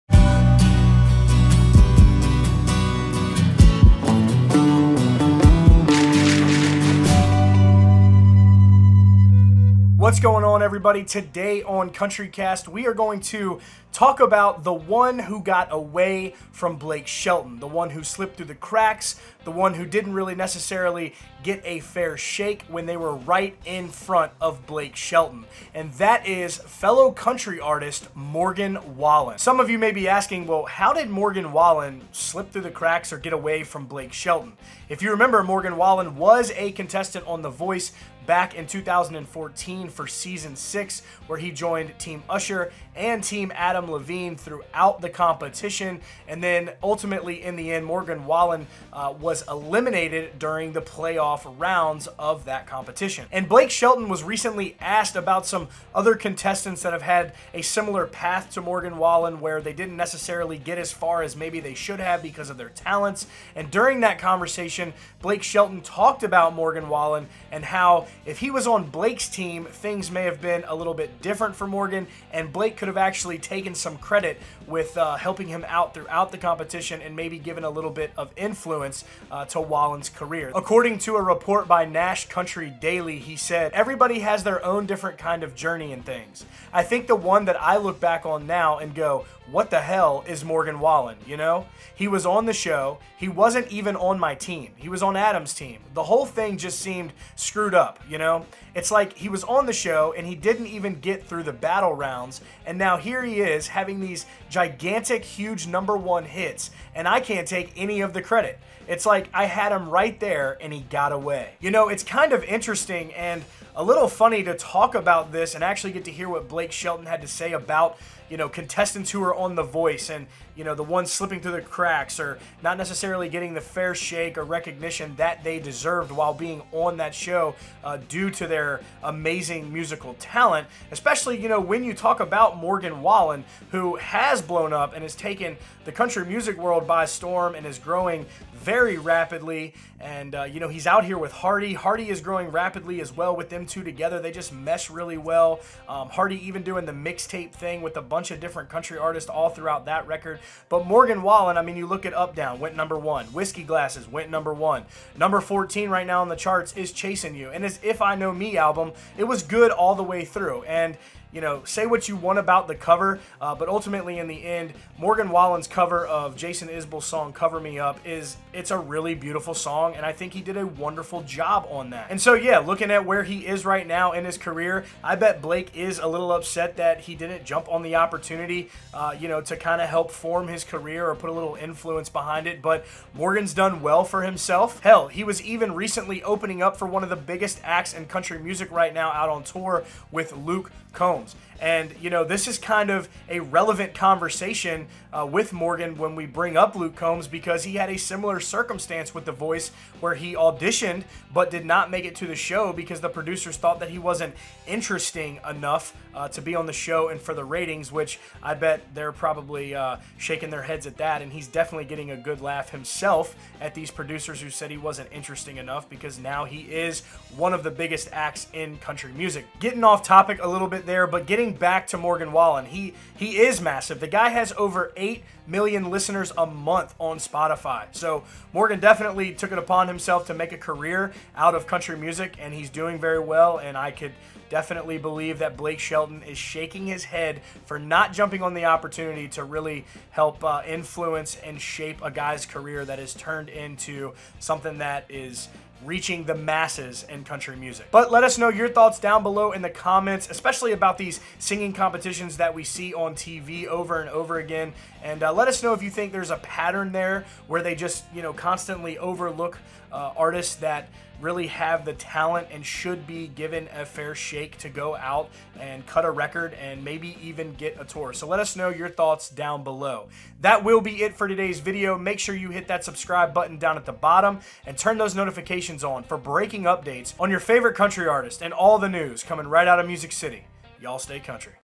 Blake Shelton Talks About The One That Got Away
Country Music Star, Blake Shelton, recently spoke about how he missed out on a very special someone, who is currently riding high in Country Music. Blake Shelton missed his opportunity with this special someone during the 6 season of The Voice, during the battle rounds. Can you remember who that may be?